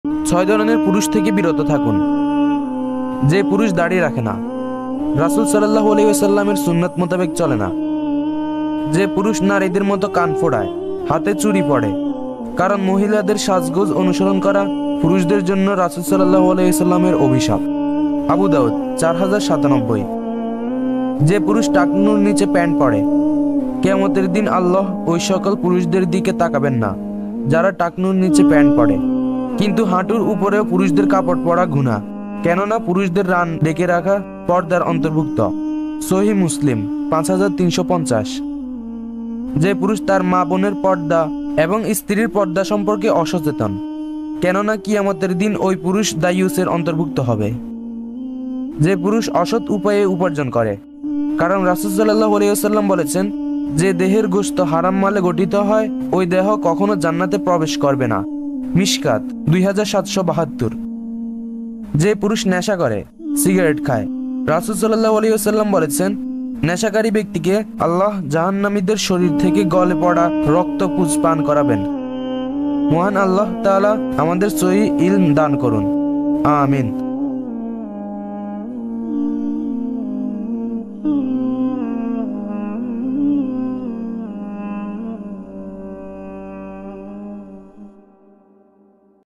৬ ধরনের পুরুষ থেকে বিরত থাকুন! Mercy Of Allah || Yeasin Arafat
৬ ধরনের পুরুষ থেকে বিরত থাকুন! Mercy Of Allah || Yeasin Arafat

১. যে পুরুষ দাড়ি রাখেনা, রাসূল (সাঃ) সুন্নাত মোতাবেক চলে না। 🖤

২. যে পুরুষ নারীদের মত কান ফোড়ায়, হাতে চুরি পড়ে। কারণ মহিলাদের সাজগোজ অনুসরণ করা পুরুষদের  জন্য রাসূল (সাঃ) এর অভিশাপ।🖤
~আবু দাউদ : ৪০৯৭

৩. যে পুরুষ টাখনুর নিচে প্যান্ট পড়ে।
( কেয়ামতের দিন আল্লাহ ঐসকল পুরুষদের দিকে তাকাবেন না যারা টাখনুর নিচে  প্যান্ট পড়ে, কিন্তু হাঁটুর উপরেও পুরুষের কাপড় পরা গুনাহ কেননা, পুরুষের রান ঢেকে রাখা পর্দার অন্তর্ভুক্ত) 🖤
~সহিহ মুসলিম : ৫৩৫০

৪. যে পুরুষ তার মা, বোনের  পর্দা ( ভবিষ্যতে স্ত্রীর) সম্পর্কে অসচেতন। কেননা,  কেয়ামতের দিন ঐ পুরুষ দাইয়্যুস এর অন্তর্ভুক্ত হবে।🖤
~ হাদিস সম্ভার : ১৭১৩

৫. যে পুরুষ অসৎ উপায়ে উপার্জন করে।
কারন রাসূল(সাঃ) বলেছেন
"যে দেহের গোশত হারাম  মালে গঠিত ঐ দেহ কখনো জান্নাতে প্রবেশ করবে না"।🖤
~ মিশকাত :২৭৭২

৬. যে পুরুষ নেশা করে,  সিগারেট খায়।
( রাসুল (সাঃ) বলেছেন নেশাকারী ব্যক্তিকে আল্লাহ জাহান্নামীদের শরীর থেকে গলে পড়া রক্তপুজ পান করাবেন)🖤
~মুসলিম : ২/১৬৭

মহান আল্লাহ তা'য়ালা আমাদের সহীহ ইলম দান করুক।(আমিন)🖤